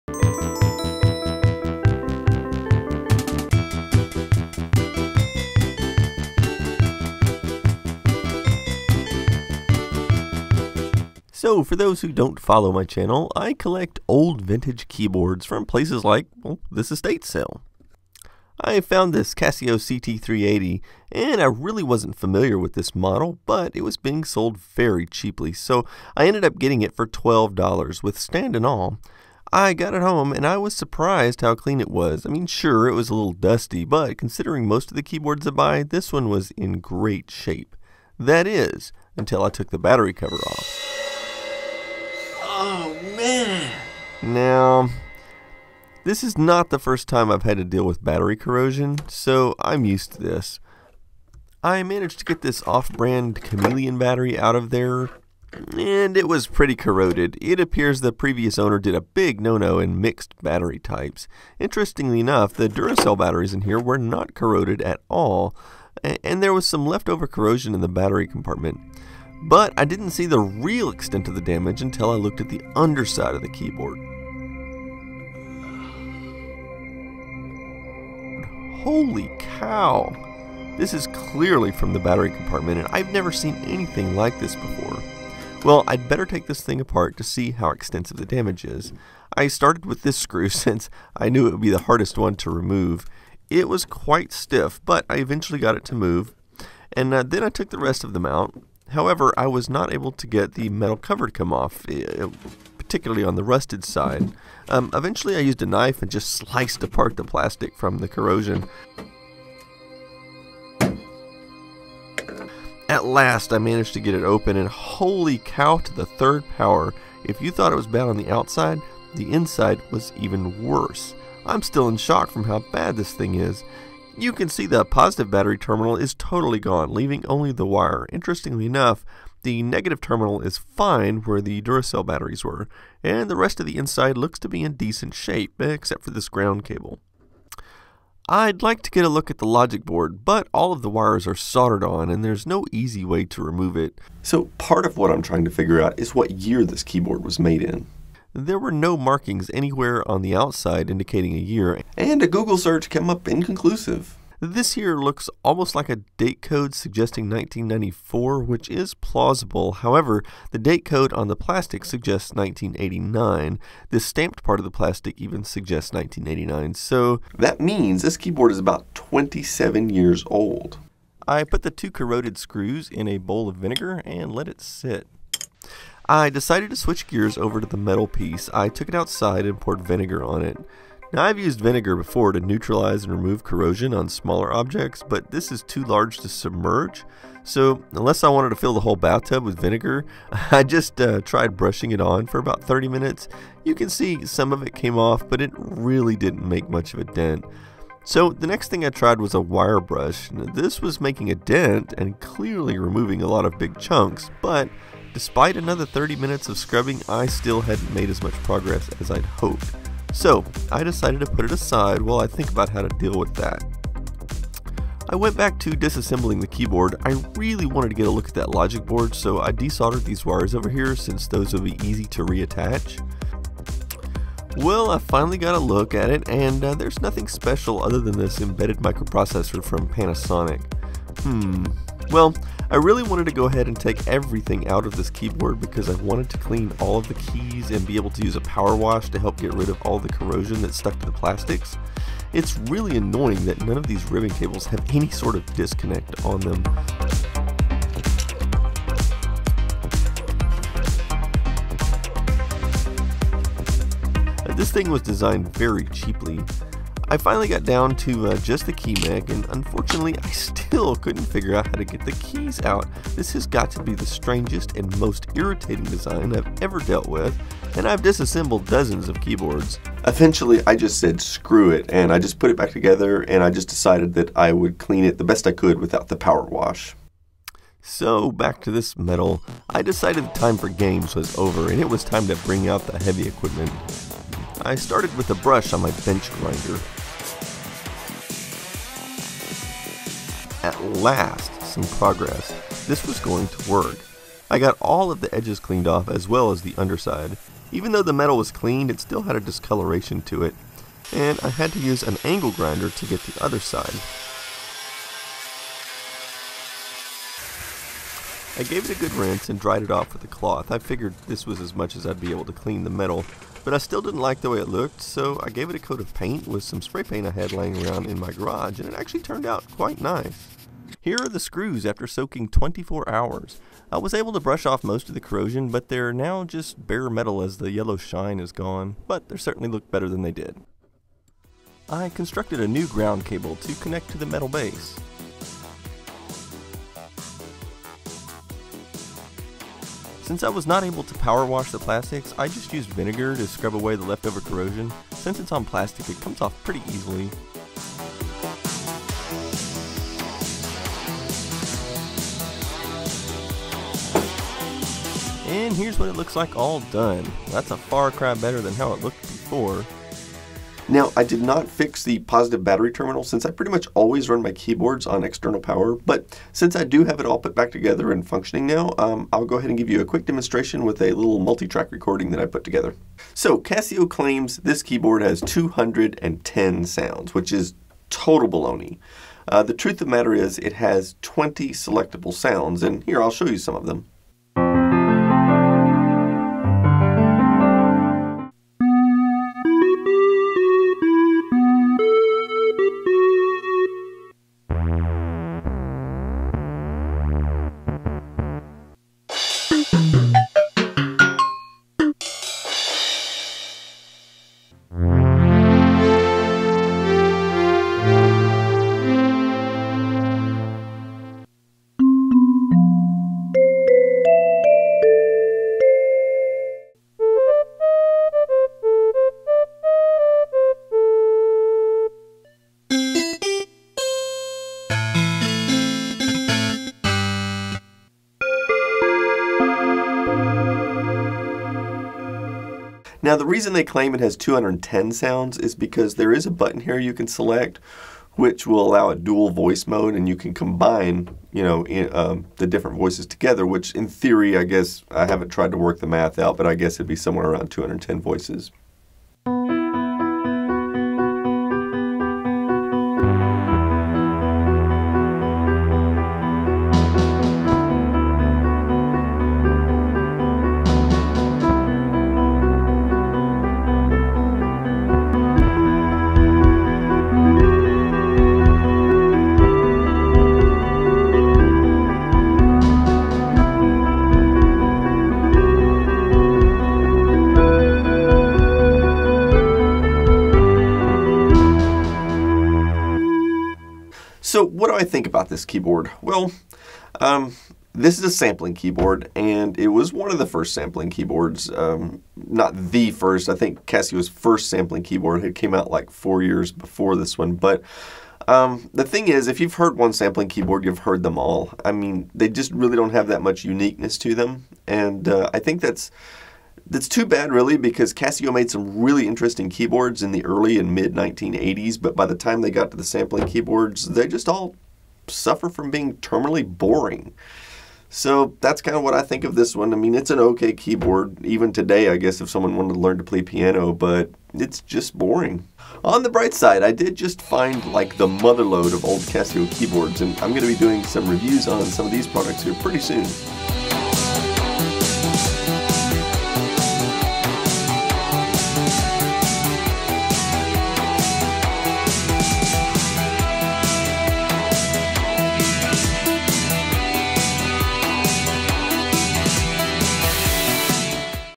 Worst battery leakage I've ever seen! Casio CT 380
In this episode, I find an old Casio keyboard at an estate sale that has been eaten alive by battery leakage.  I spend some time doing a full restoration on it and then a quick demonstration.